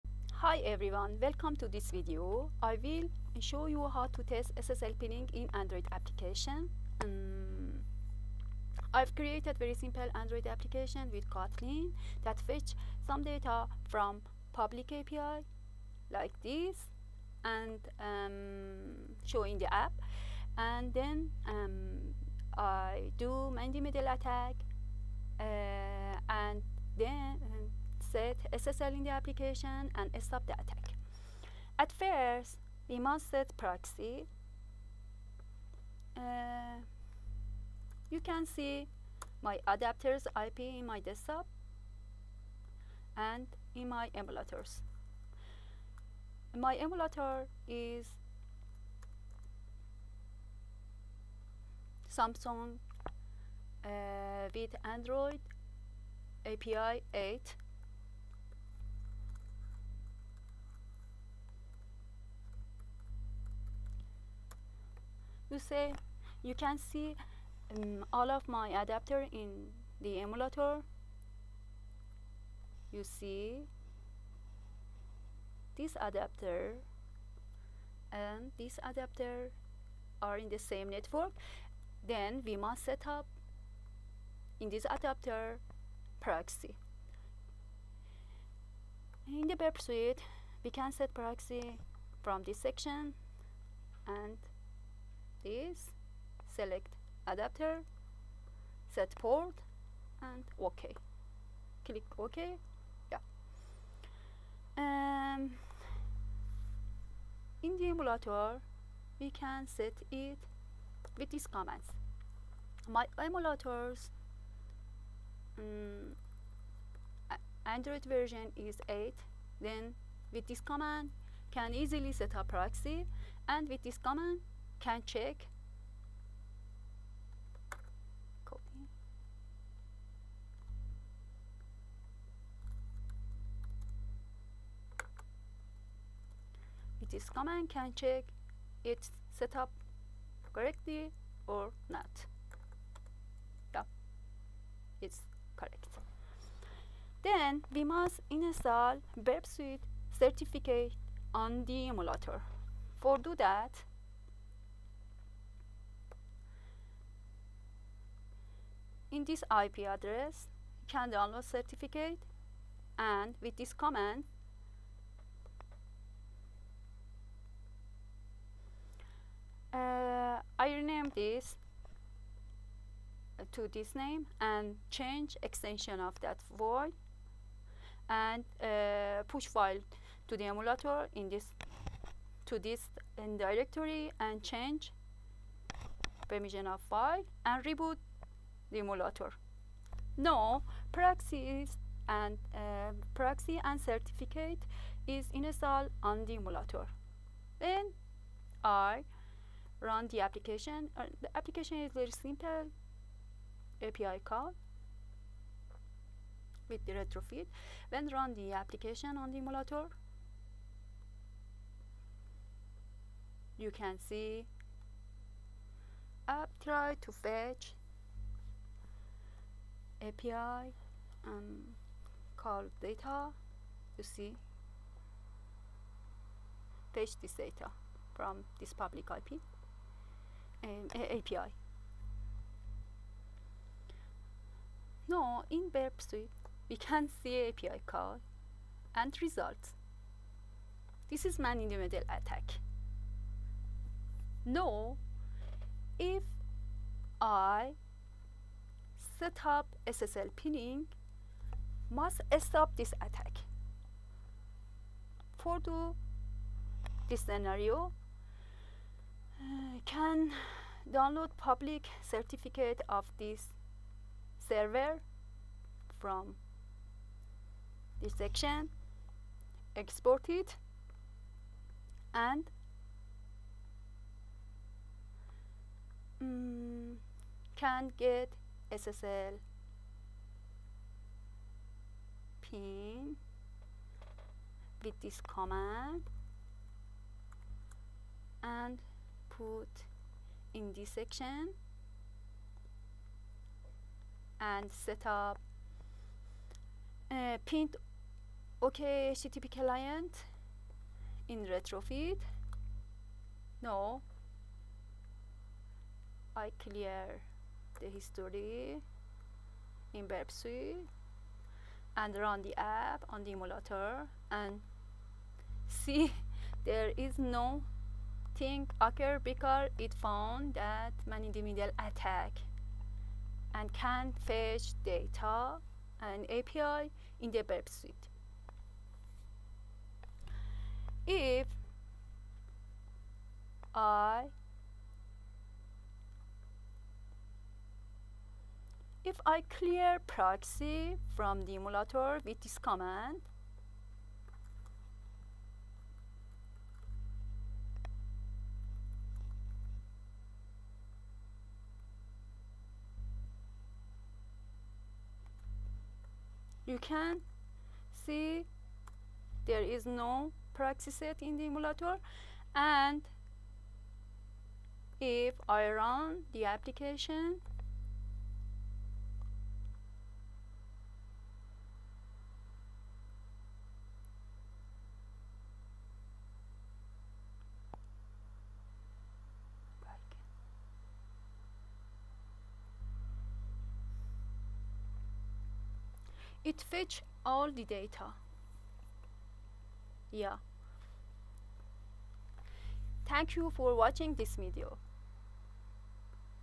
SSL Pinning in Android
🔐 Stop Man-in-the-Middle (MITM) Attacks with SSL Pinning in Android
In this video, I’ll walk you through securing an Android app using SSL Pinning — a crucial technique to defend against MITM attacks and protect sensitive user data.

• How to set up and use Burp Suite to intercept Android network traffic
• How to configure a proxy on an Android emulator
• How to test your app’s vulnerability to MITM attacks
• How to implement SSL pinning using OkHttpClient and CertificatePinner
• How SSL pinning protects your app by validating server certificates

👨‍💻 About the Demo App
I’ve built a simple Android app in Kotlin that fetches data from a public API using Retrofit. Initially, the app is vulnerable to interception — but after implementing certificate pinning, Burp Suite can no longer capture the traffic.

🛠️ Tools & Technologies Used
• Kotlin + Retrofit + OkHttp
• Android Emulator (Genymotion)
• Burp Suite
• OpenSSL
• SSL Certificate Pinning

✅ Whether you're an Android developer or a mobile security enthusiast, this video is a practical step-by-step guide to strengthening your app’s security.